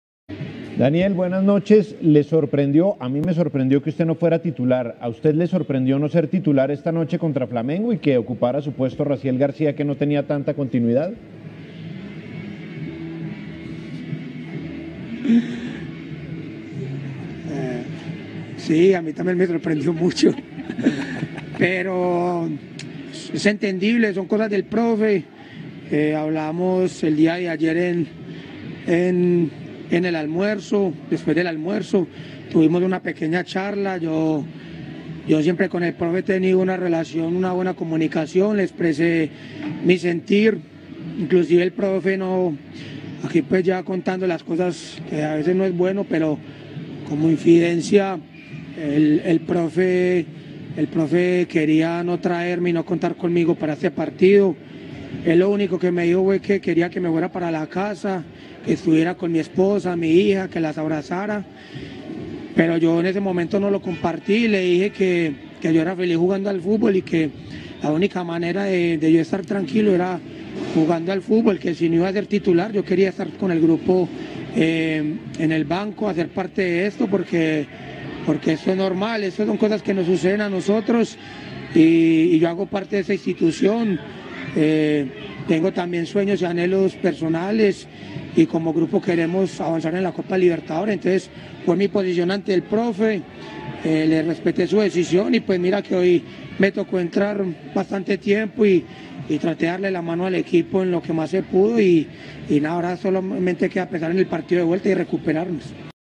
La sorpresiva confesión de Daniel Cataño en entrevista en vivo con ESPN
El volante ‘pijao’ rompió su silencio luego de haber errado el penal en la final de la Liga BetPlay.

Imágenes de ESPN.